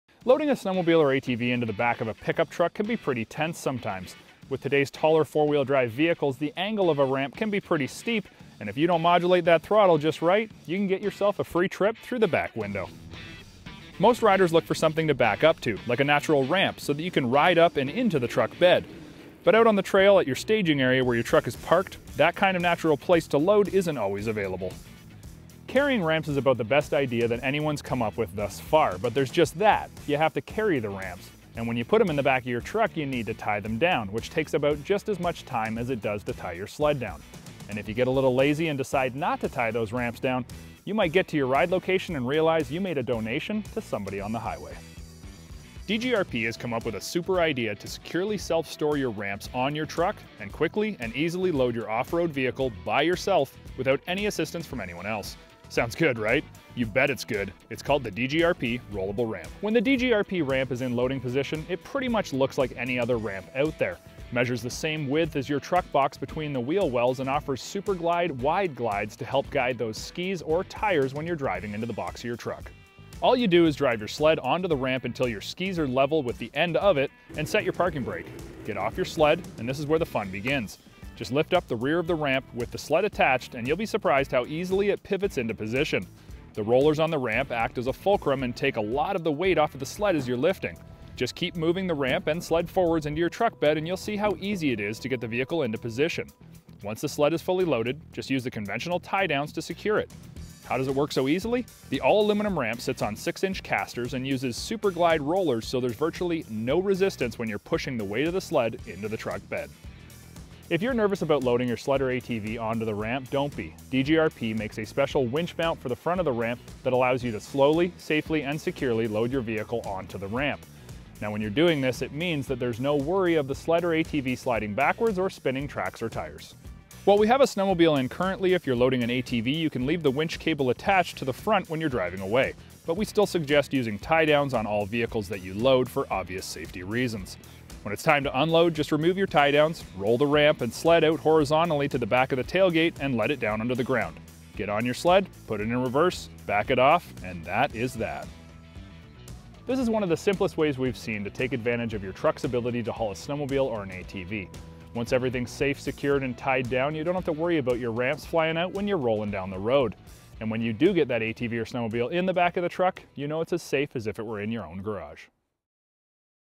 Maybe The Best Way To Load Your Sled
AJ looks at an innovative new way to load and unload your motorized toys into the back of your pick-up truck. This ingenious ramp and loading system, by DGRP, saves valuable time and effort so you can get your machine on the ground faster and get back on the road quickly when you’re all done playing.